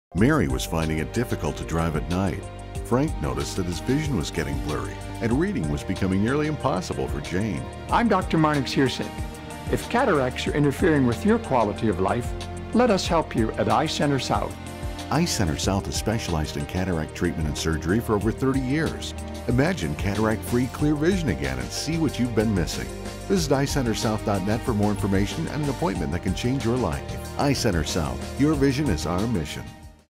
Cataract Care in Dothan - Eye Center South
Experiencing changes in your vision? Our physicians are dedicated to providing personal patient care and the latest options for cataract patients.

Marnix E. Heersink, M.D., Cataract and Laser Refractive Surgeon
Zsolt C. Bansagi, M.D., Cataract and Laser Refractive Surgeon
William L. Bennett, M.D., Cataract Surgeon, General Ophthalmology
Sebastian B. Heersink, M.D., Cataract and Laser Refractive Surgeon

to schedule an evaluation. Early detection and treatment of cataracts is key.

Your Vision...Our Mission.